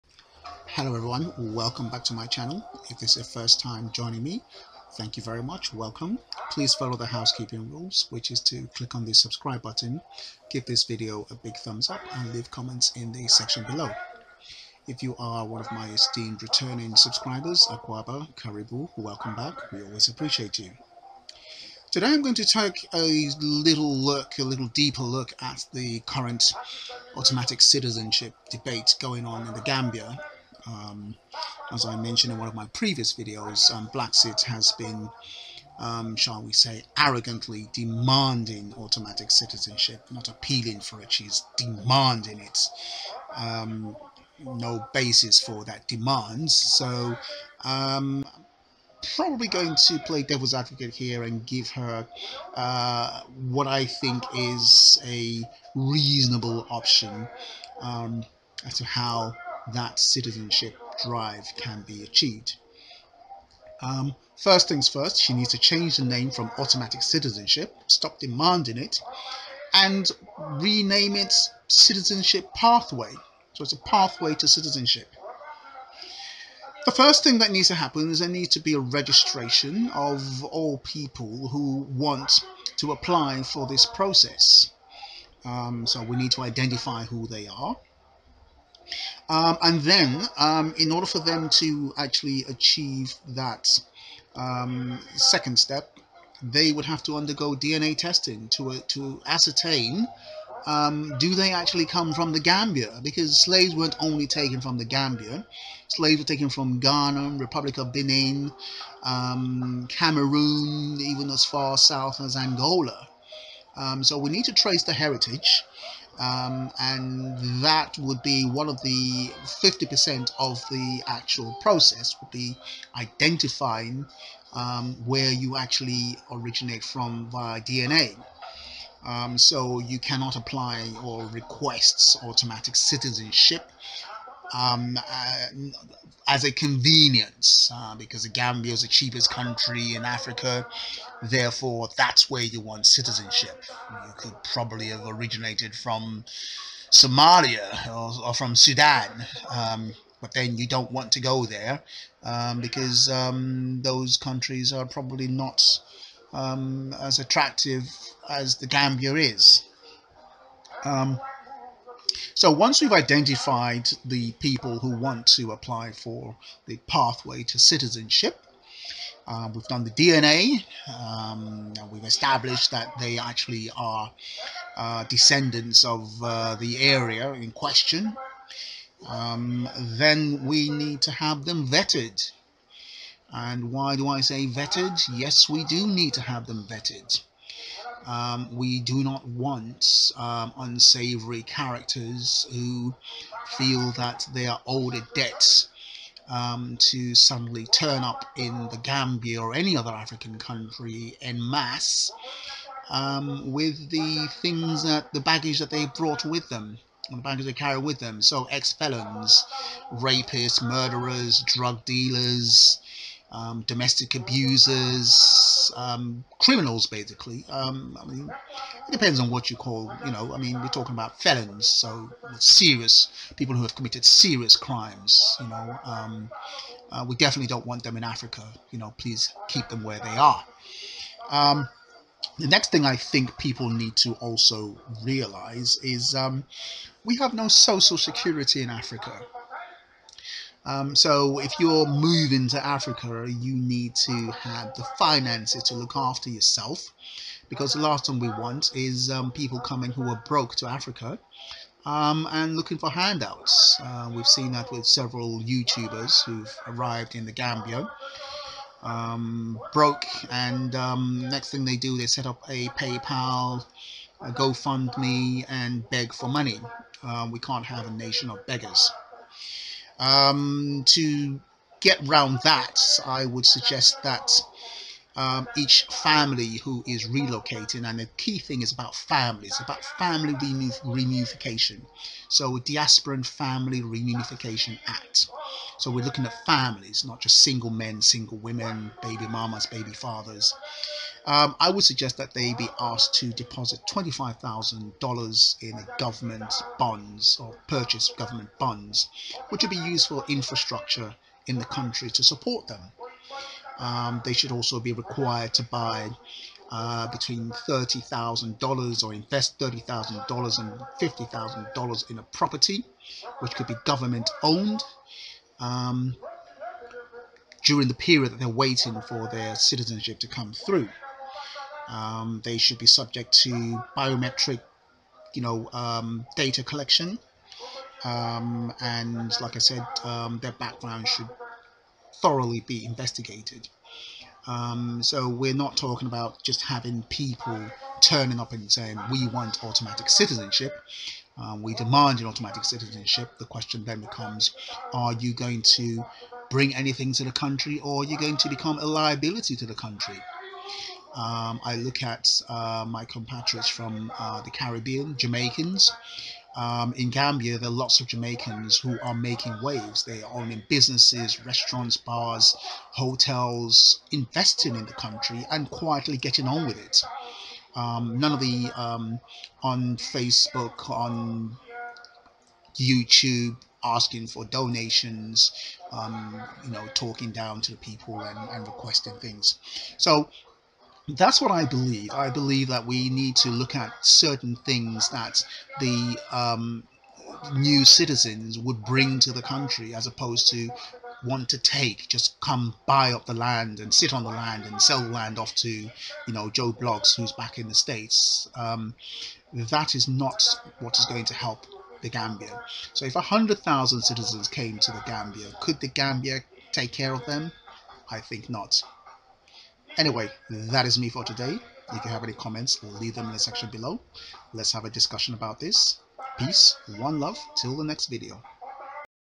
How to get Citizenship in Africa - Pathway to Citizenship in Africa - What Diasporans can expect.
Today, I give my thoughts on possible pathways to citizenship, for members of the diasporan community.
It is a reasonable process, and one that creates a win win scenario, for both the country, and the new citizens.
What do you think about this proposed Pathway to Citizenship for Diasporans?